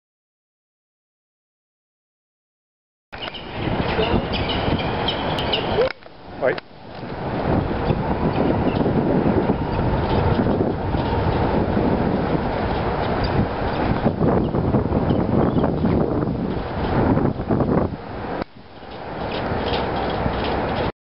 Algo et Polly chassent un autre faucon pèlerin à l'Échangeur Turcot!
Samedi 14 juin 2014, 15h45.